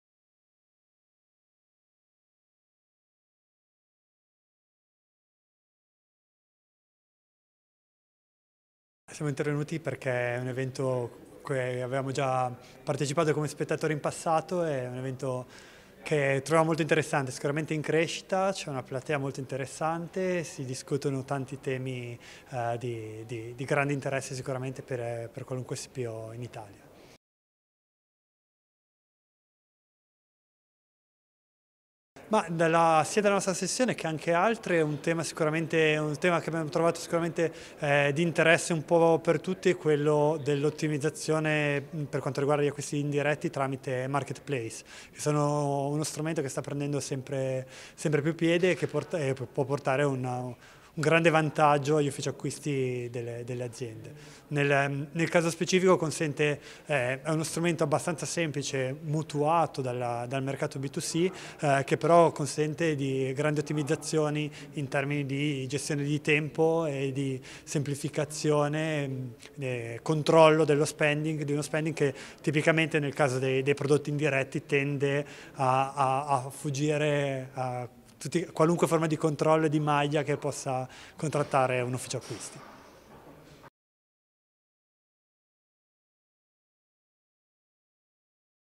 Intervista a Giorgio Luciani, Country Manager di Markit Italia
A Procurement Forum 2020 abbiamo intervistato Giorgio Luciani, Country Manager di Markit Italia.
Guarda la video intervista!